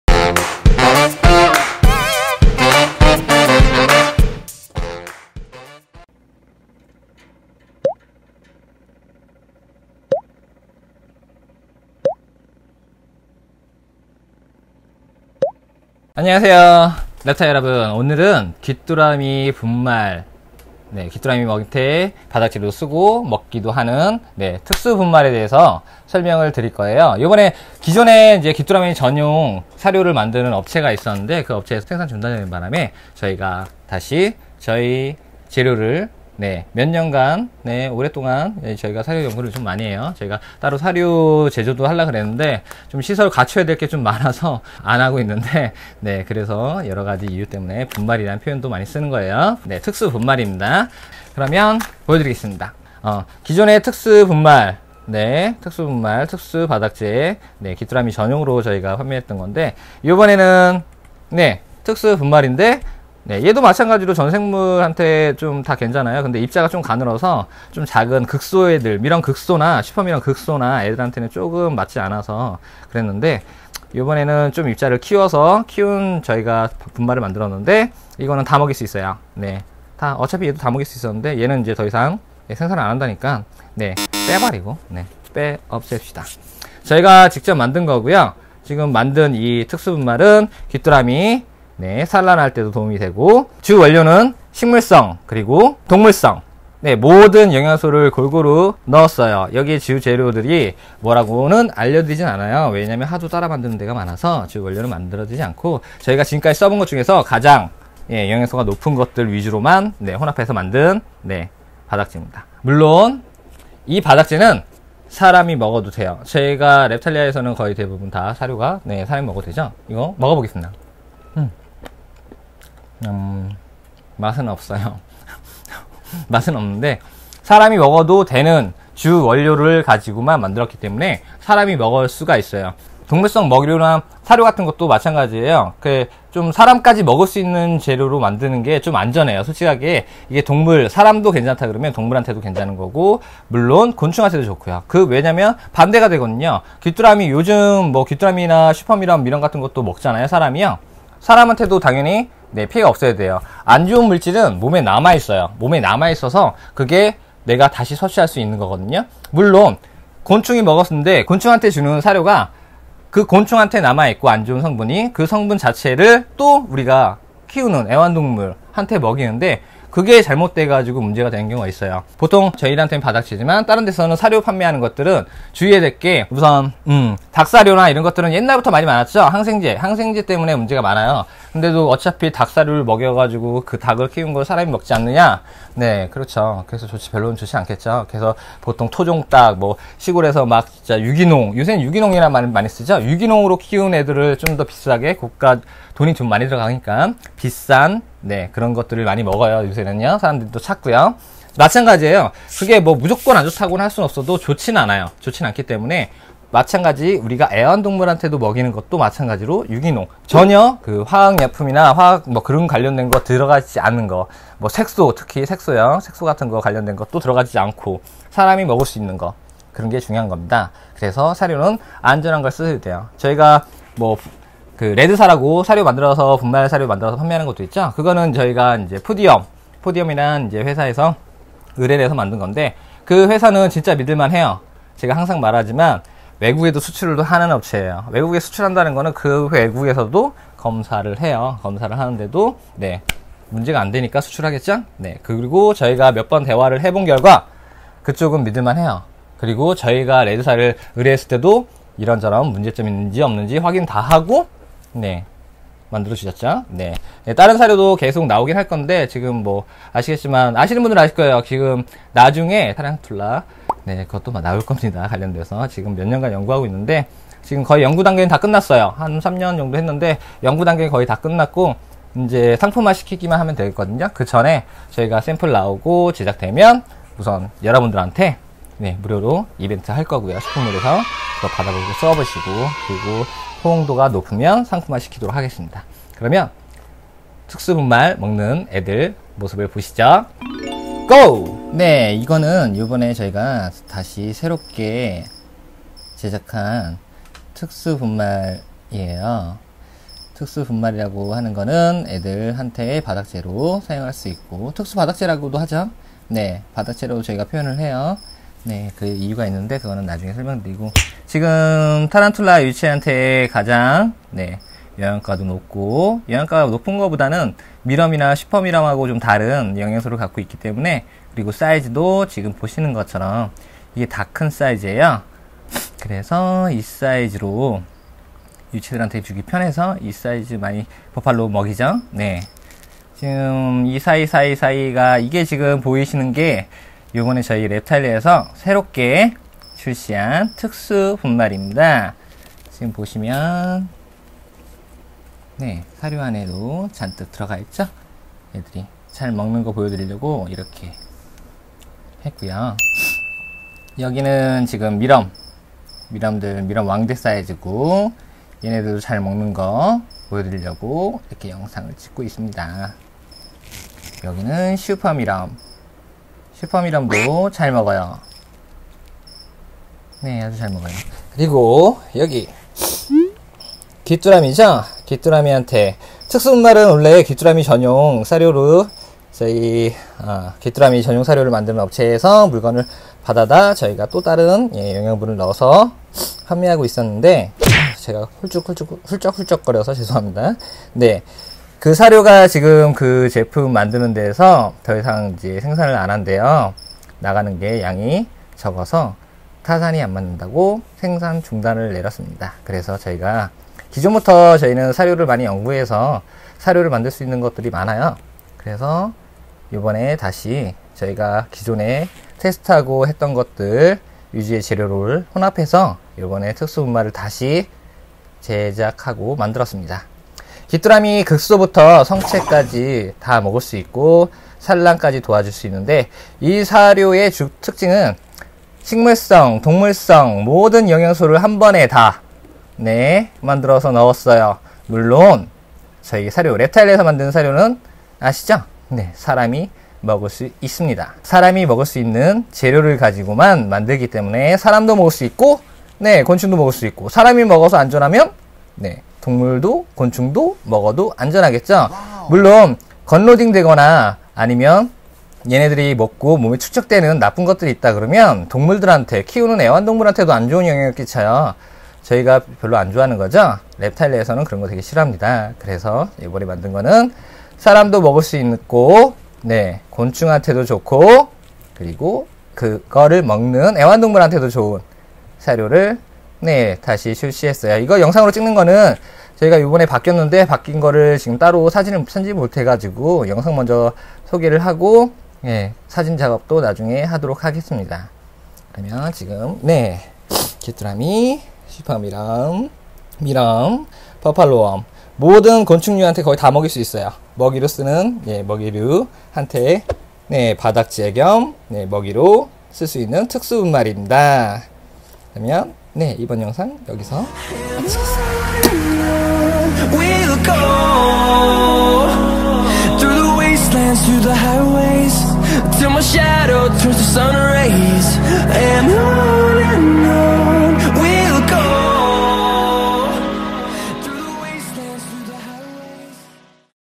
특수분말, 귀뚜라미,슈퍼밀웜, 밀웜 전용 분말 - 사람이 먹을 수도 있는 안전한 재료로 구성된 제품
기존 특수분말 변경 - 동물성 & 식물성 혼합형 분말 (최고의 영양 공급)